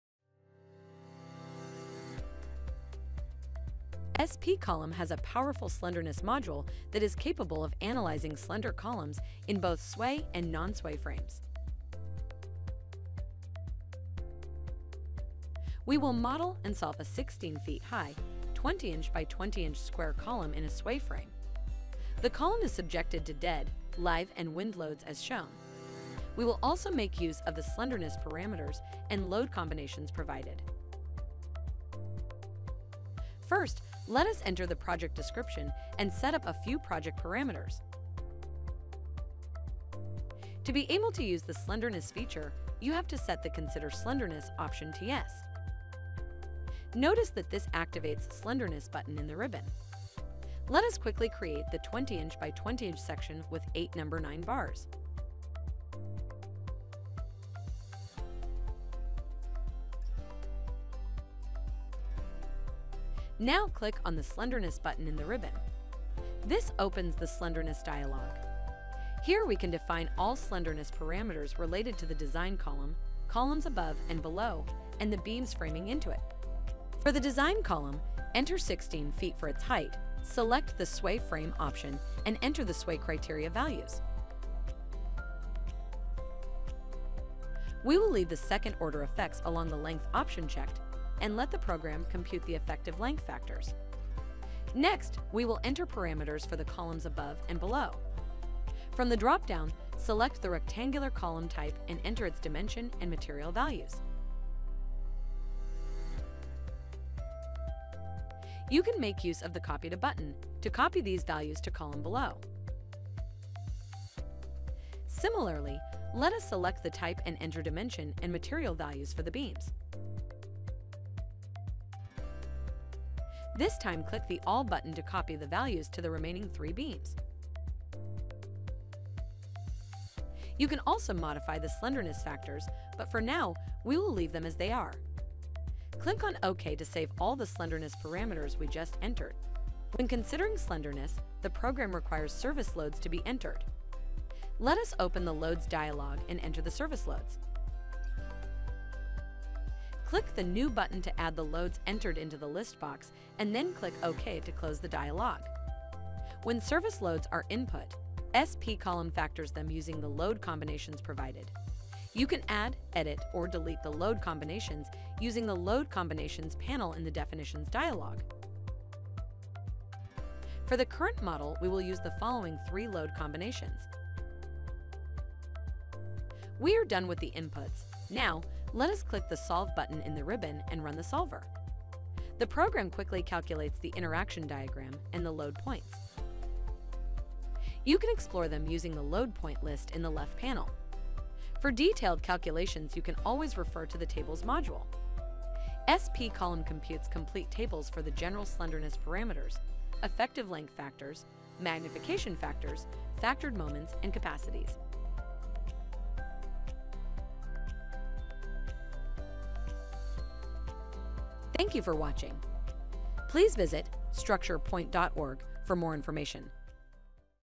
Slenderness Considerations In spColumn
Examine column slenderness and stability in sway and non-sway frames and the impact of second order (P-Delta) effects on moment magnification using spColumn's powerful slenderness module.